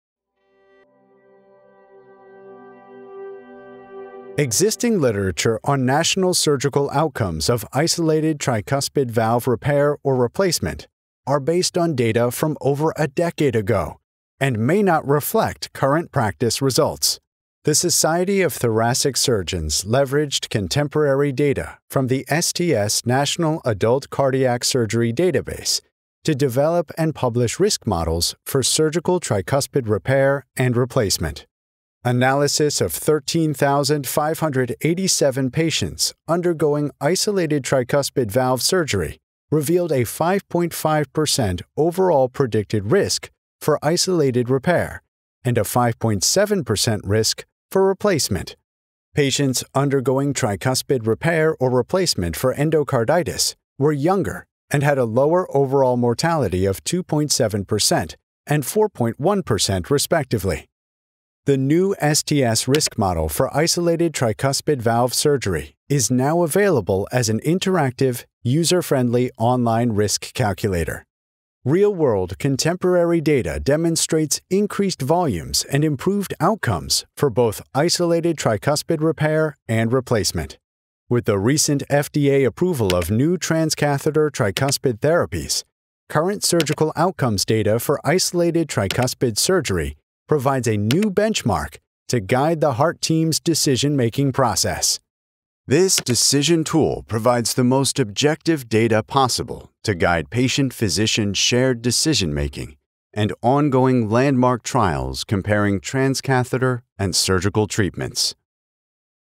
Isolated Tricuspid Valve Surgery Risk Calculator
The expanding use of transcatheter valve technologies has changed the landscape in the treatment of valvular disease. Valve surgery is rapidly shifting to more complex scenarios that involve multiple valve interventions and concomitant procedures. To inform heart team decision-making, The Society of Thoracic Surgeons has launched a surgical risk calculator on isolated tricuspid valve repair and replacement. This new tool leverages contemporary data from the STS Adult Cardiac Surgery Database.